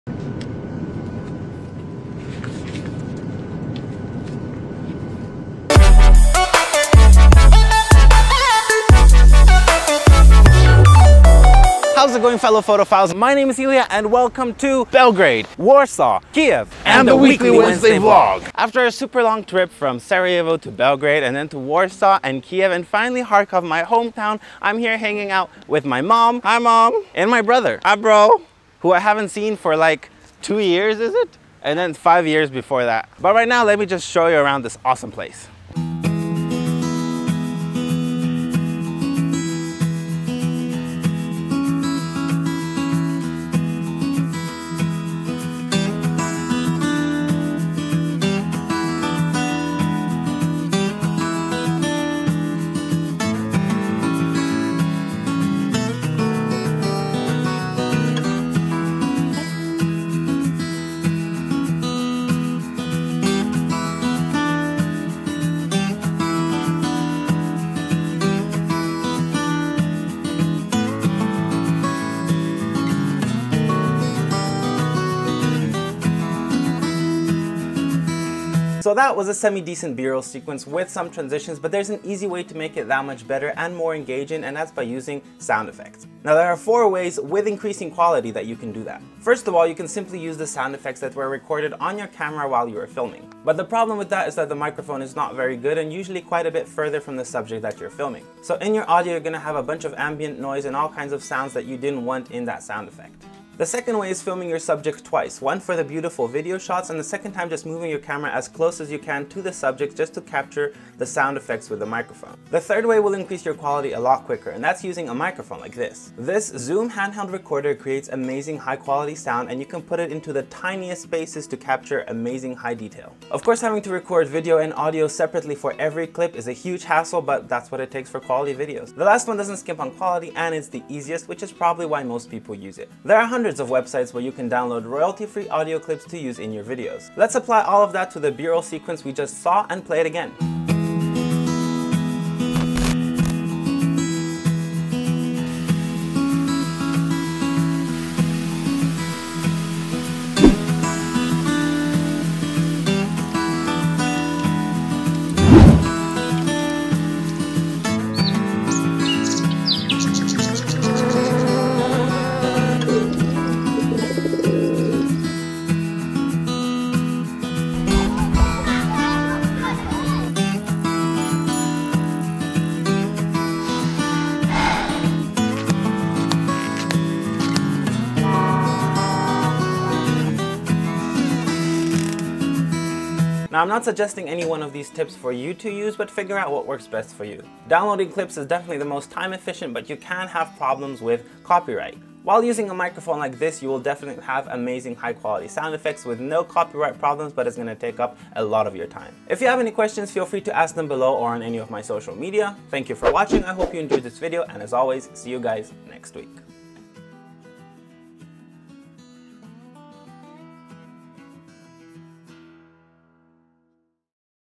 Make B-roll WAAY more engaging with SOUND EFFECTS
Yesterday was mourning day for the genocide in Srebrenica, and in honor and respect of the victims and their families, the vlog was moved to today.

B-roll may look great, but it's still not nearly as engaging as it could be if you add sound effects. Vlogging is a competative market, and as a vlogger you should be trying to use every tool at your disposal to stand out from the competition.

My stuff:

18-105mm f3.5-5.6 (very adaptable)
50mm f1.4 (killer lens)